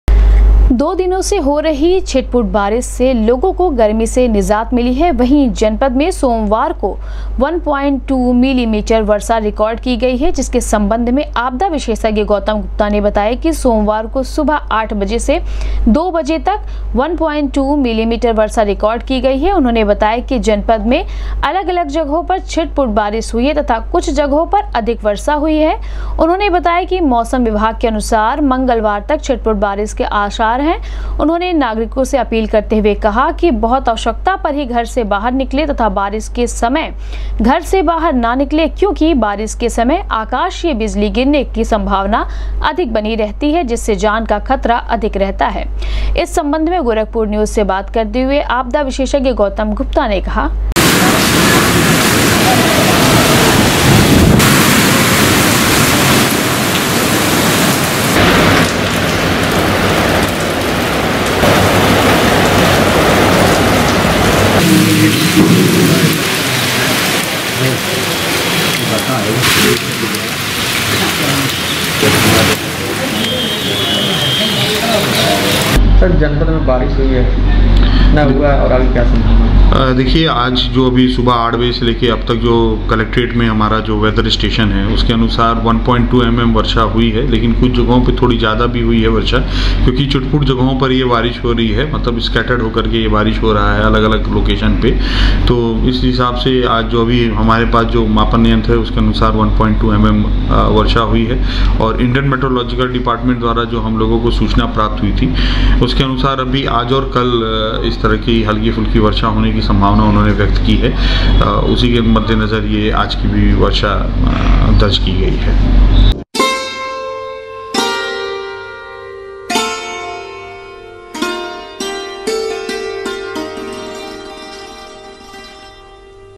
दो दिनों से हो रही छिटपुट बारिश से लोगो को मिली गर्मी से राहत, आपदा विशेषज्ञ ने कहा,मंगलवार तक है
दो दिनों से हो रही छिटपुट बारिश से लोगो को मिली गर्मी से राहत, आपदा विशेषज्ञ ने कहा,मंगलवार तक है छिटपुट बारिश के आसार